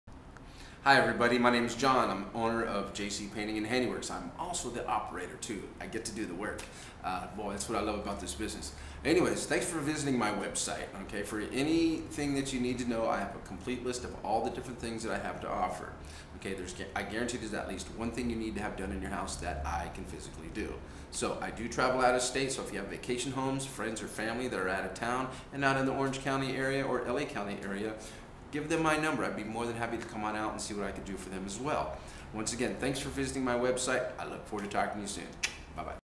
Home Imrovement Expert John Coombes
Home Imrovement Expert John Coombes introduces his website